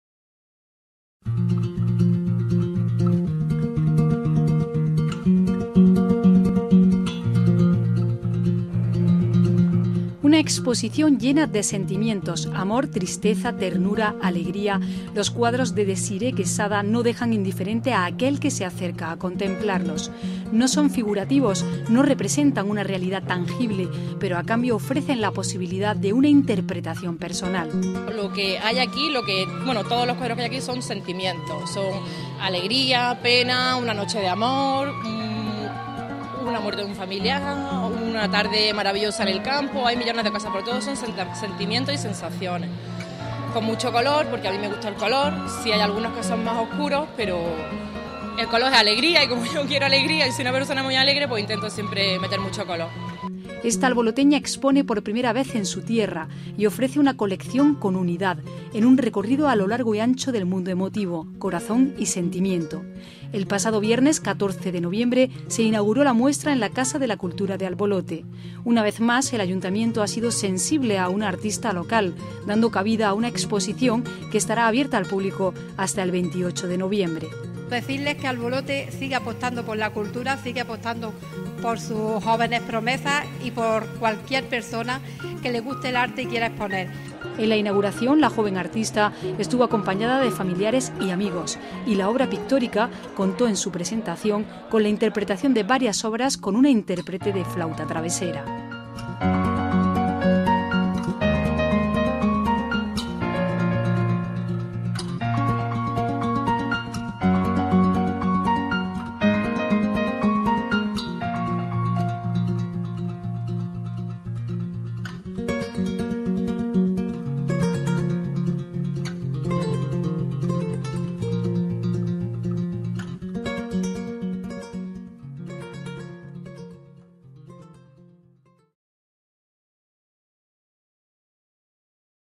EXPOSICIÓN DESIREE QUESADA
Inauguración de la exposición de pintura de la alboloteña Desiree Quesada el pasado 14 de noviembre de 2014 en la sala de exposiciones de la Casa de la Cultura de Albolote.